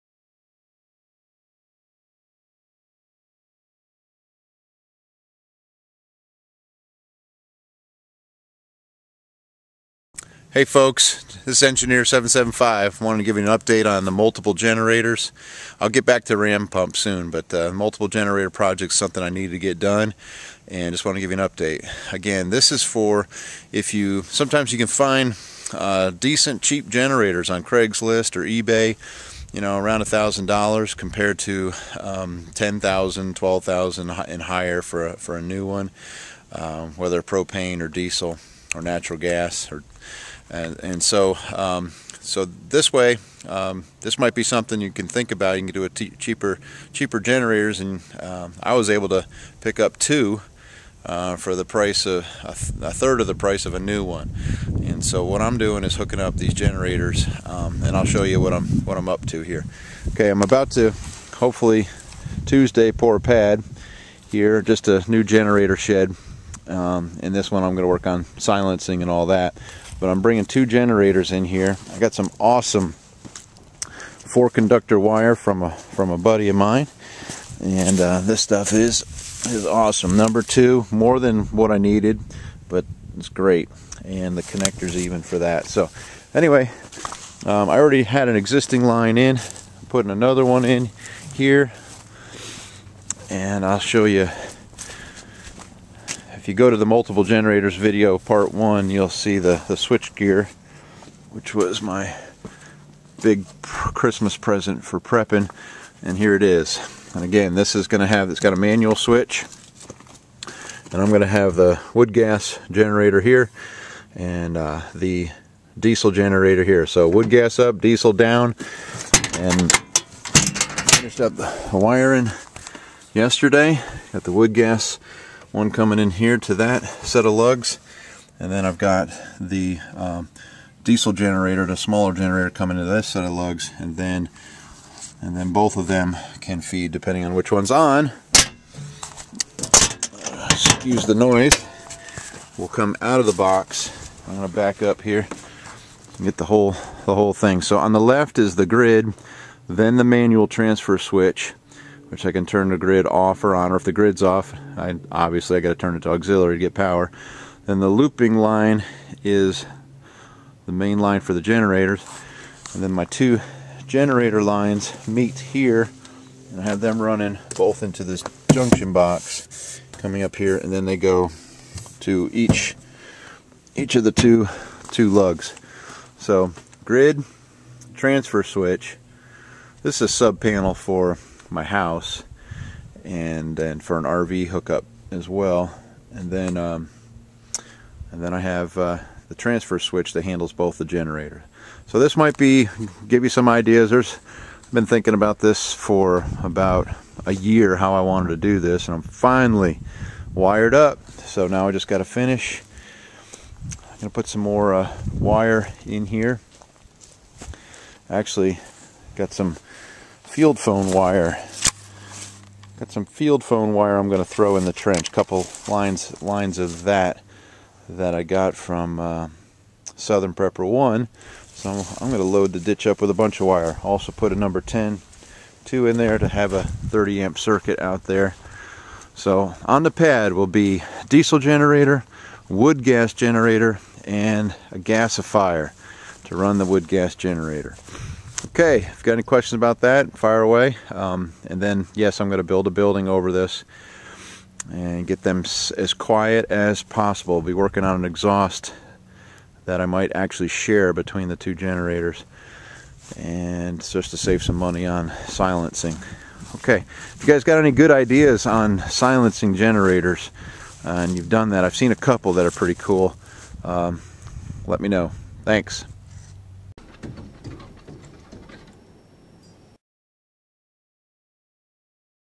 Multiple Generators Pt. 2
The next phase in getting 2 backup power sources connected to the homestead.  Fuel sources:  Diesel, Vegetable oil, Propane, Wood